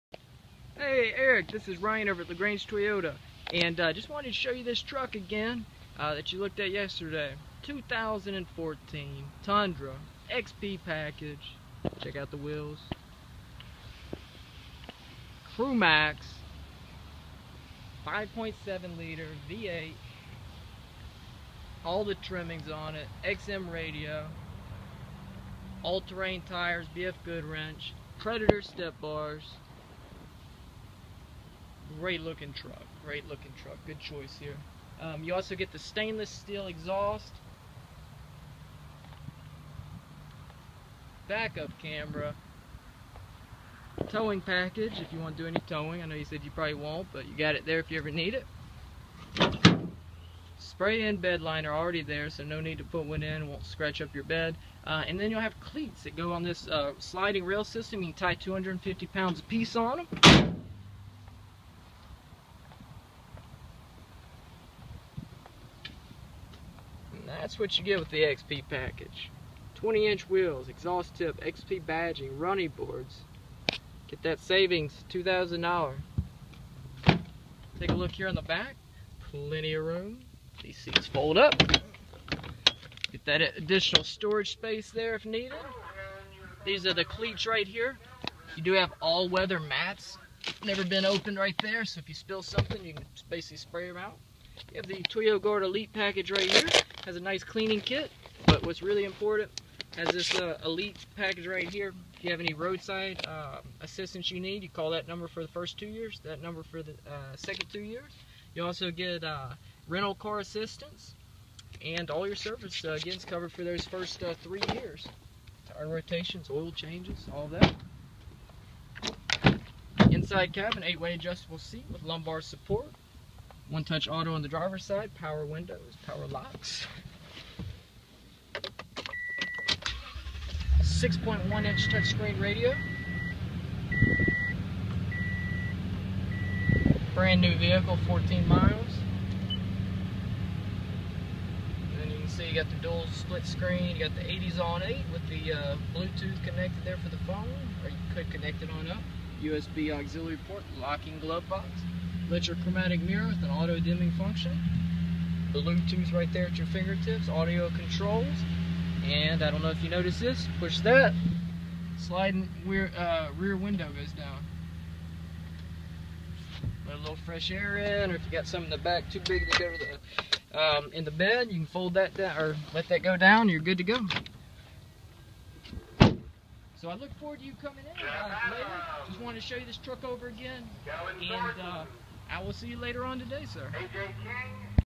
2014 Toyota Tundra XP Review
Demonstration of 2014 Toyota Tundra XP by Ryan Lane at LaGrange Toyota